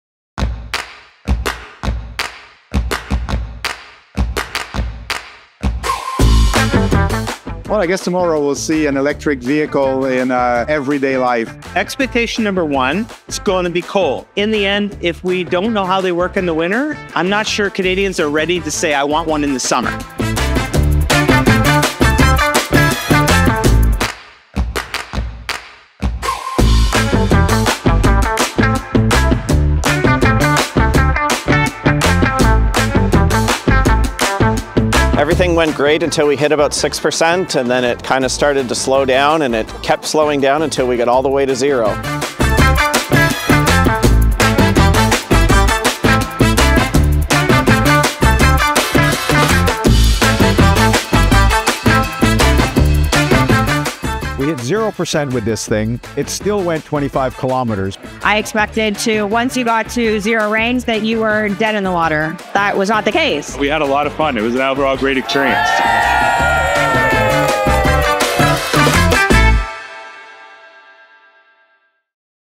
The Ultimate EV Winter Stress Test
CAA conducts road test of EV range, charge in winter conditions.

Vehicles representing more than two-thirds of Canadian EV sales were driven from Ottawa to Mont Tremblant in sub-zero temperatures.